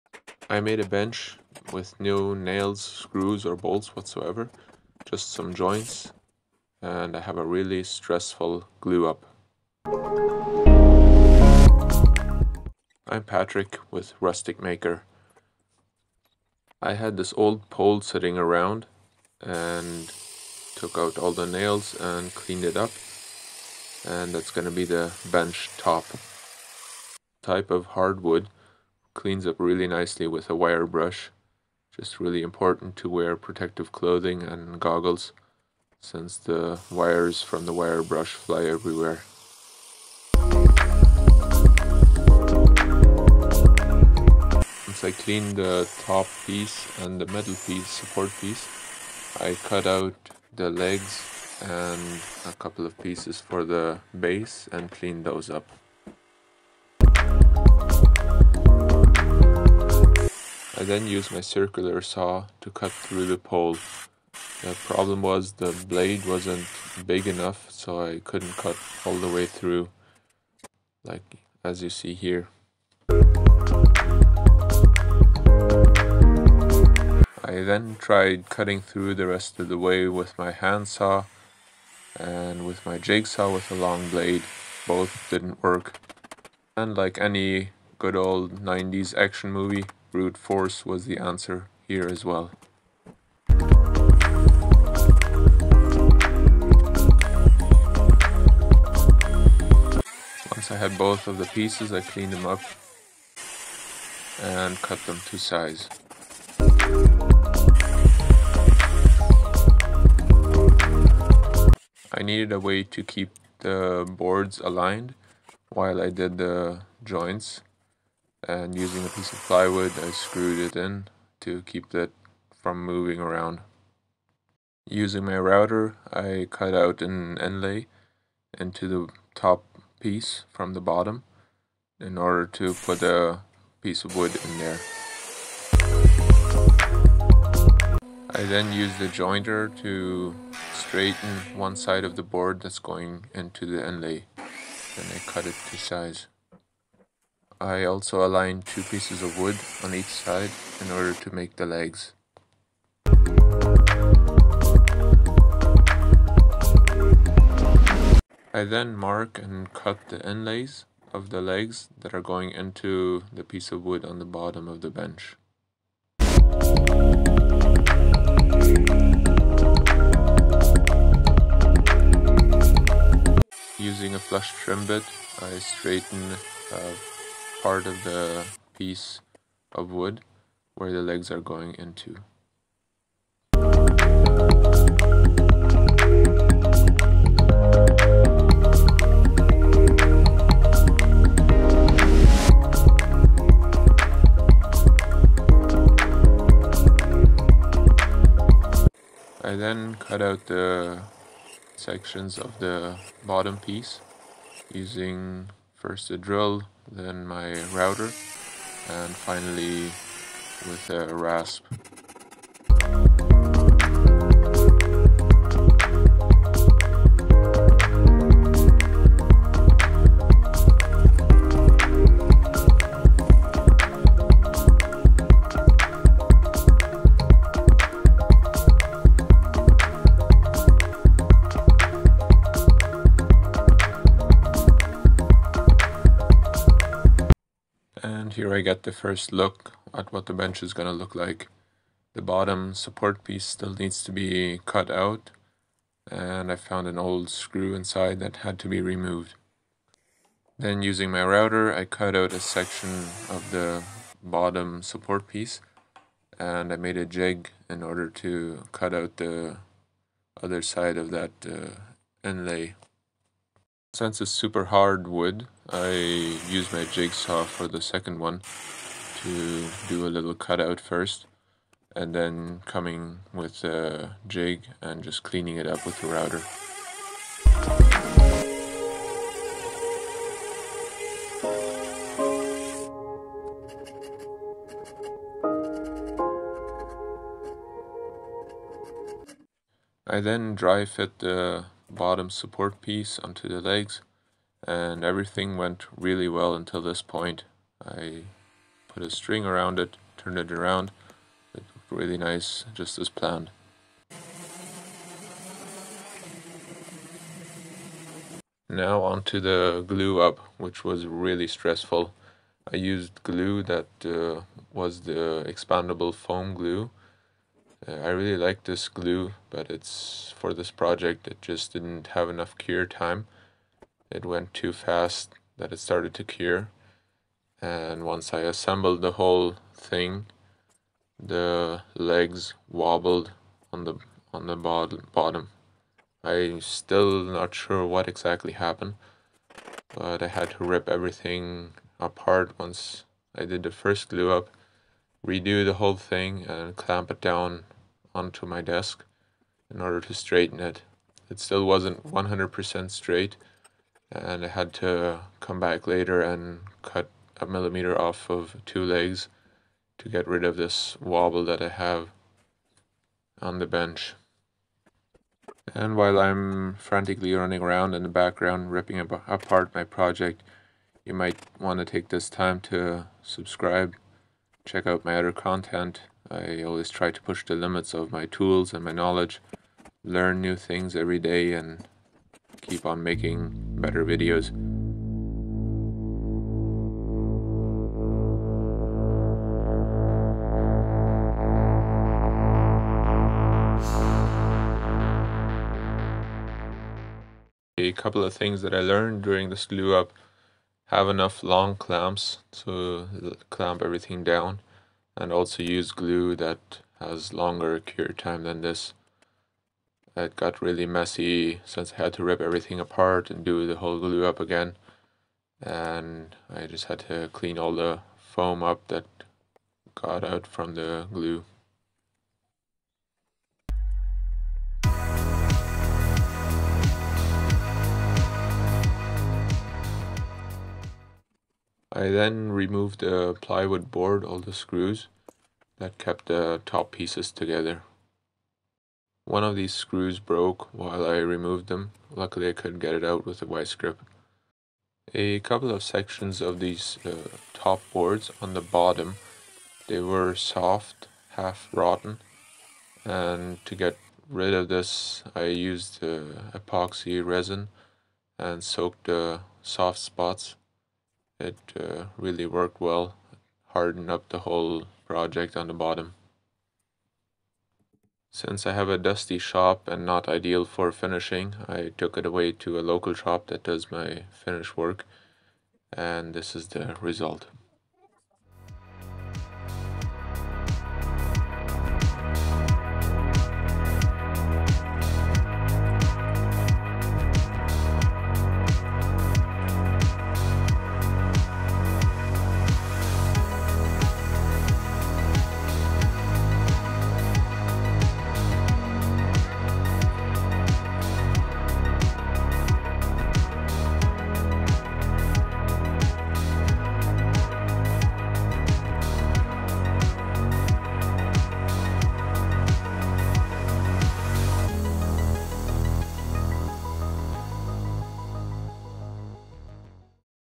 Rustic Bench Idea | No Nails, No Screws. Just Joints and Glue.
I made a Bench with Reused lumber I had laying around. I didn’t use any nails, screws or bolts. Just some cut out joints and glue. The project was going great until I got to the glue up part. It was stressful.